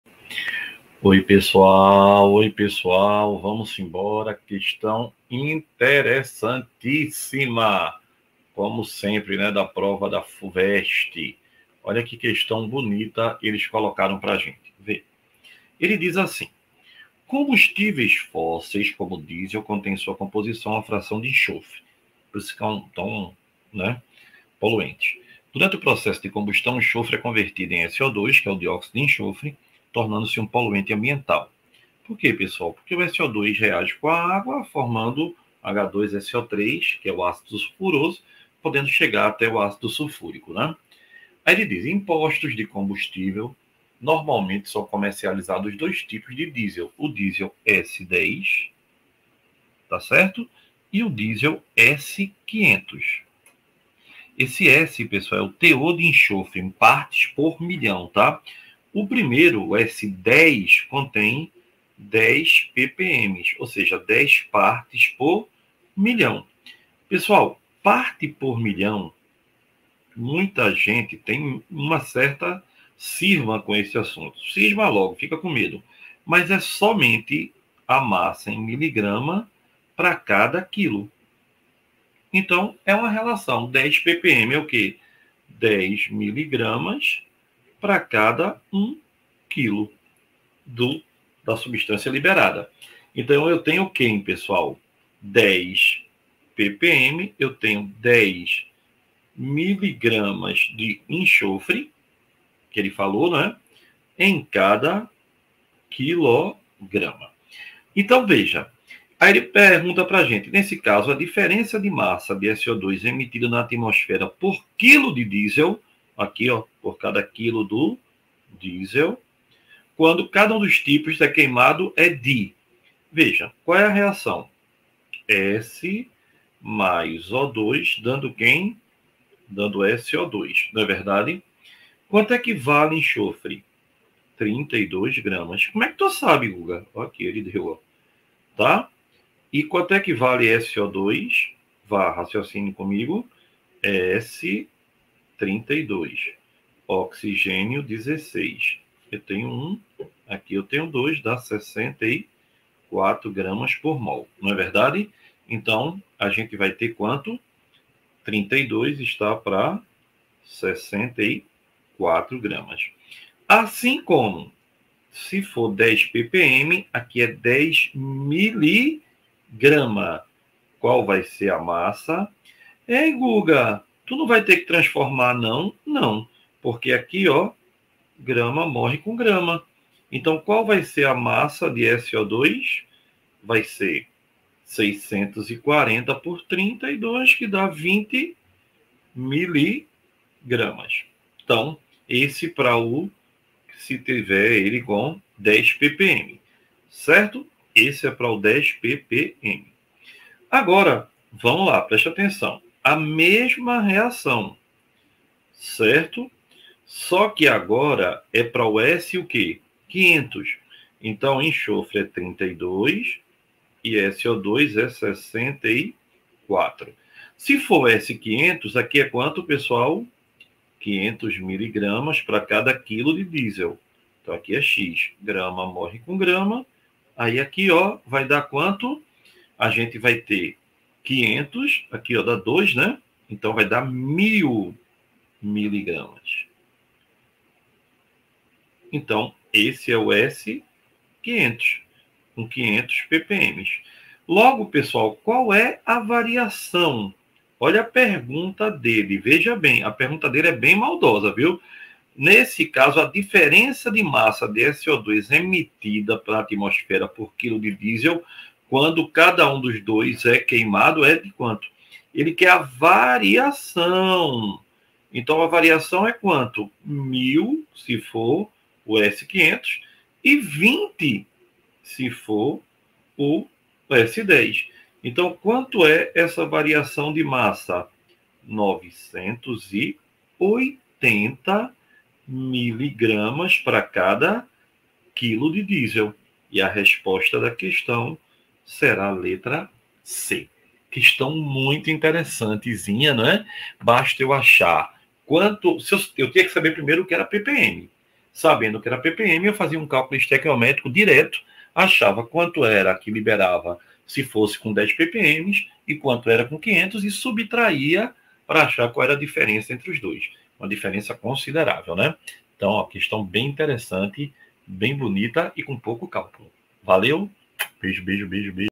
Cálculo estequiométrico - Fuvest 2023 -Combustíveis fósseis, como o diesel, contém em sua composição
(Fuvest 2023) Combustíveis fósseis, como o diesel, contém em sua composição uma fração de enxofre. Durante o processo de combustão, o enxofre é convertido em SO2, tornando-se um poluente ambiental. Em postos de combustível, normalmente são comercializados dois tipos de diesel, o Diesel S10 e o Diesel S500. O primeiro contém 10 ppm de enxofre, e o segundo, 500 ppm de enxofre.
Considere que na combustão do diesel, todo enxofre seja convertido em SO2, conforme reação a seguir: